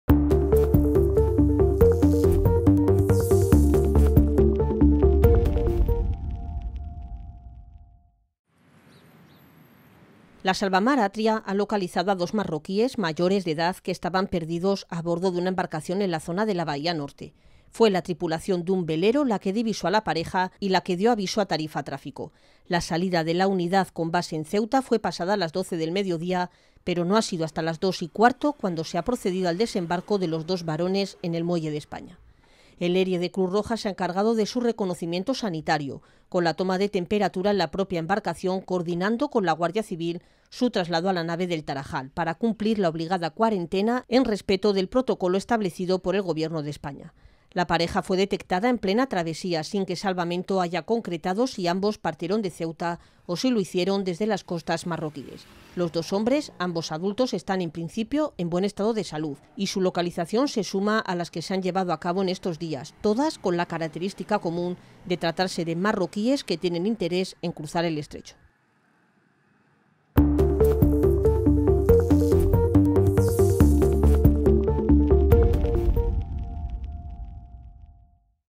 La tripulación de un velero localiza a dos inmigrantes en pleno cruce del Estrecho
La Salvamar Atria ha localizado a dos marroquíes, mayores de edad, que estaban perdidos a bordo de una embarcación en la zona de la bahía norte. Fue la tripulación de un velero la que divisó a la pareja y la que dio aviso a Tarifa Tráfico, desde donde se coordinó el operativo. La salida de la unidad con base en Ceuta fue pasadas las doce del mediodía pero no ha sido hasta las dos y cuarto de esta tarde cuando se ha procedido al desembarco de los dos varones en el muelle de España.

Imagen: Cristian Marfil